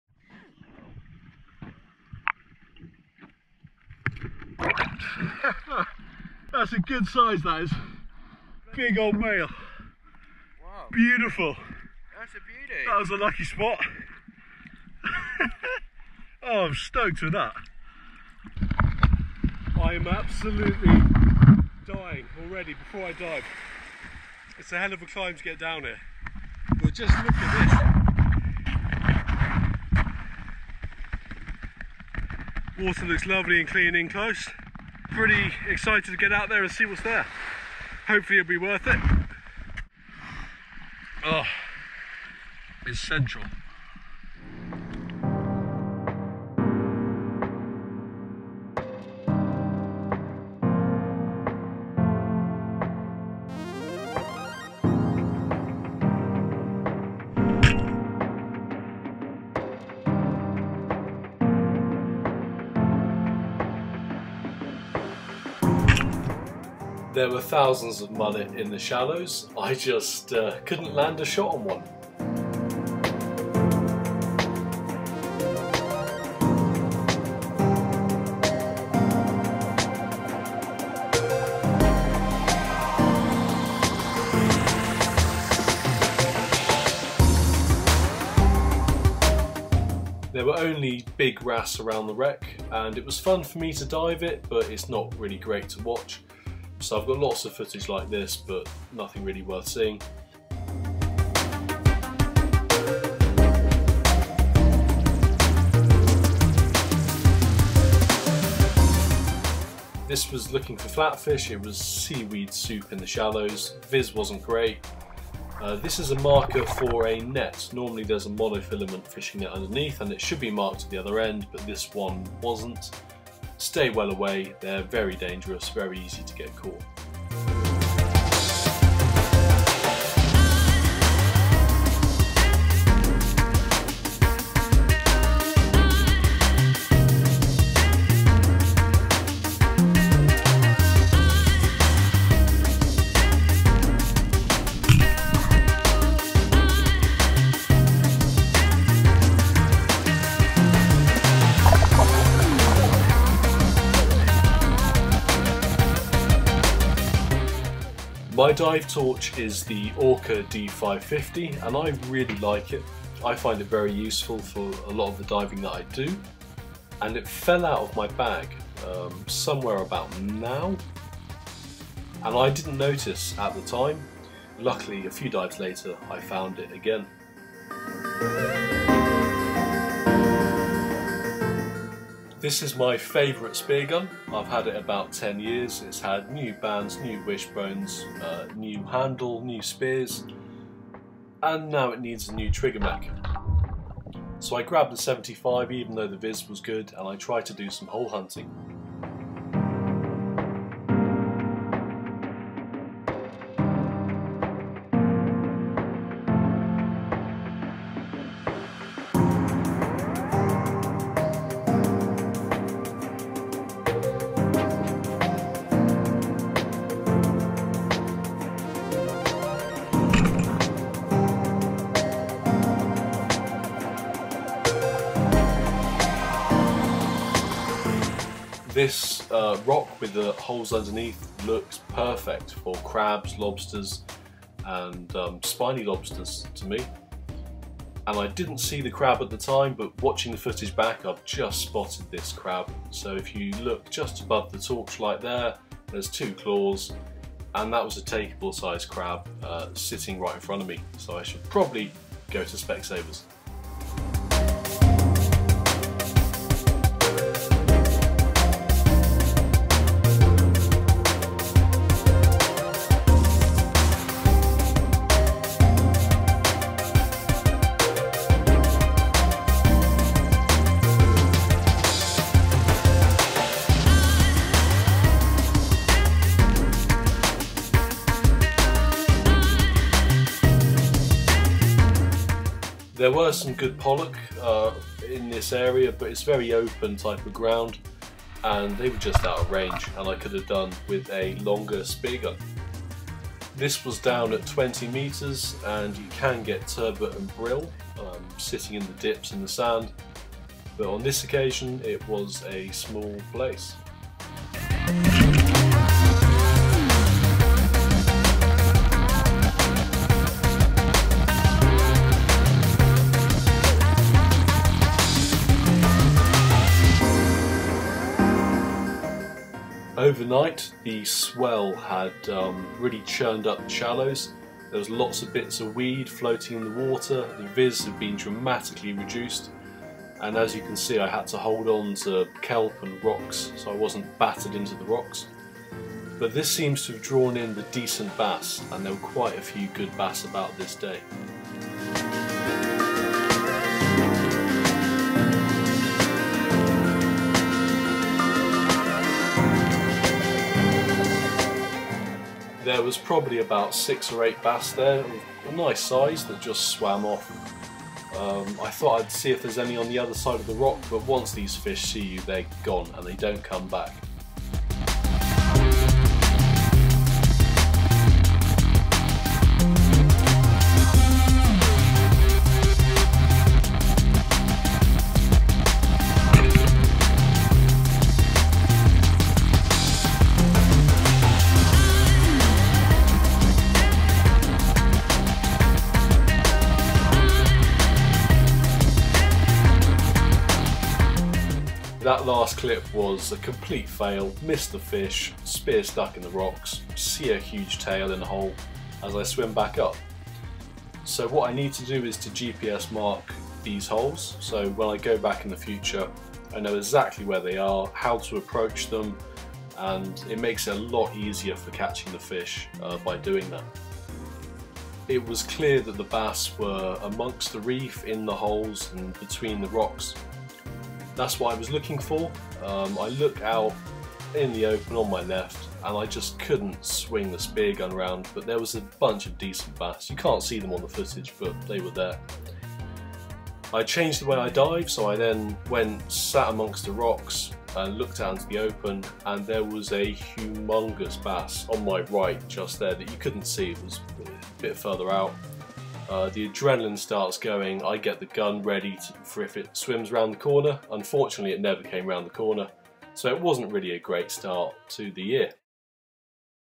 Spearfishing UK April 2022
After months out the water I had nearly forgotten how to spearfish. It was a bad start to the season but it's coming back to me slowly! It was great to be diving again and sourcing my own seafood sustainably.